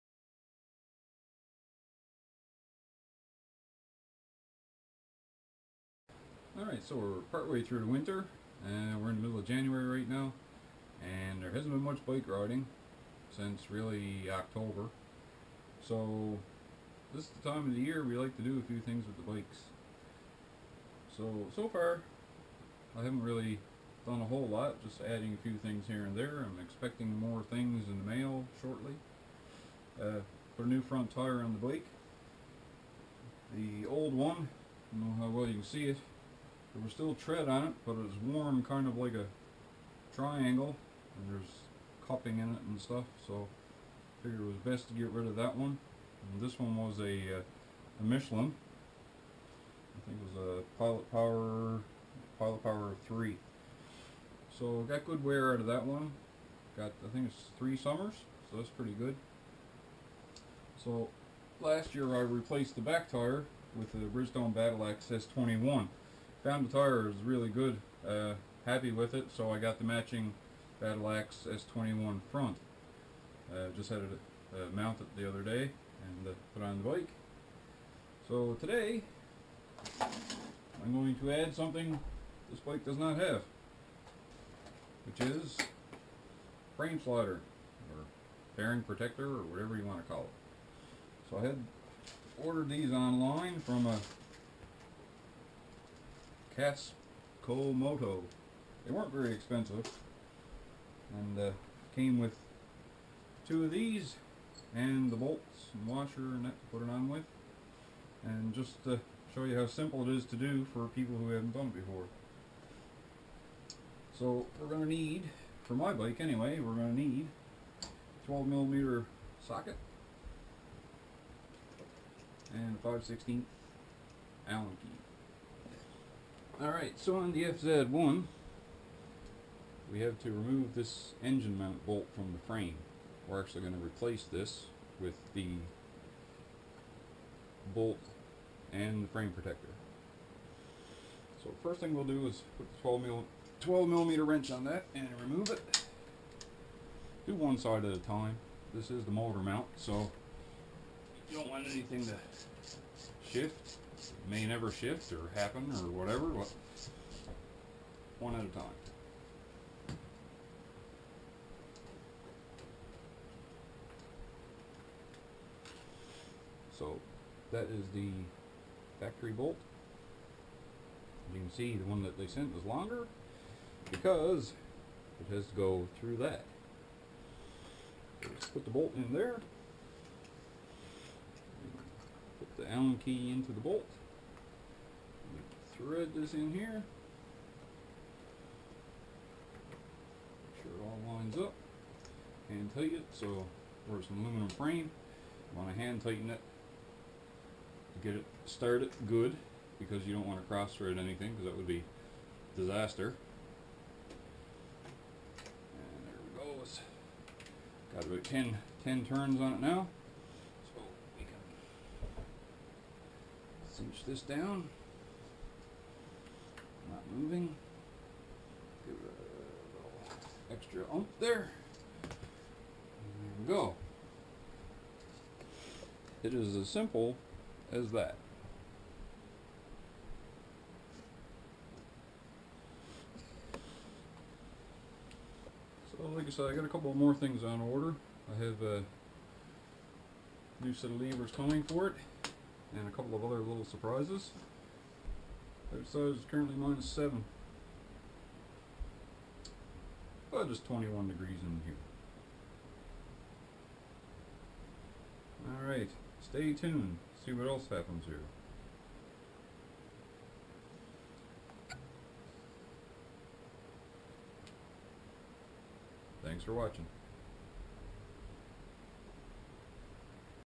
Frame Slider Install
Installing Frame Sliders in the Yamaha FZ1. Pretty Simple. Here's How.